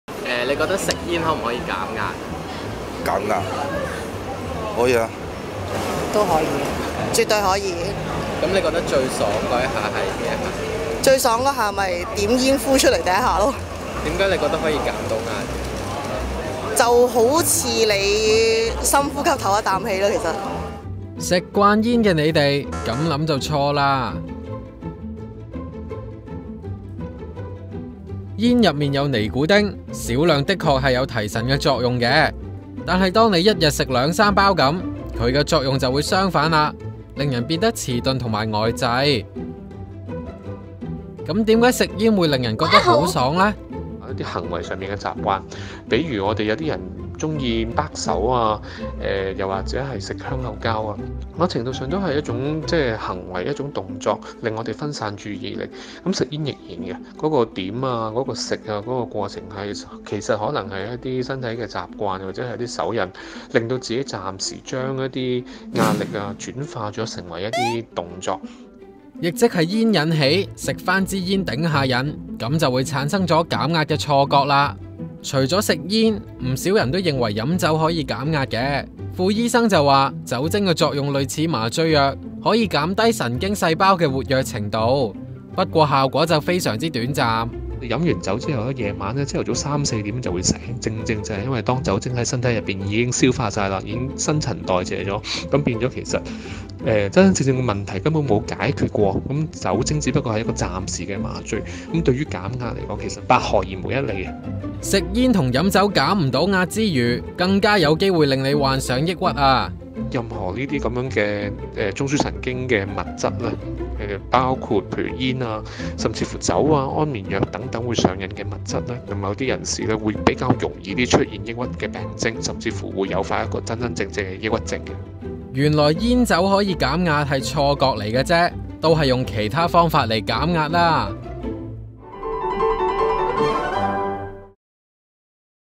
食煙飲酒減唔到壓 仲可能誘發抑鬱症？
「點起香煙飲啖酒，所有不快都溜走！」這樣想就大錯特錯，因為長期吸煙有機會令人患上抑鬱症！精神科醫生傅子健指，香煙的主要成份尼古丁，有一種很特別的特性，低劑量的確有提神作用，是興奮劑，但當「食到煙駁煙，一日食兩三包」，它的作用就會相反，變成中樞神經的抑壓劑，令人變得遲鈍和呆滯。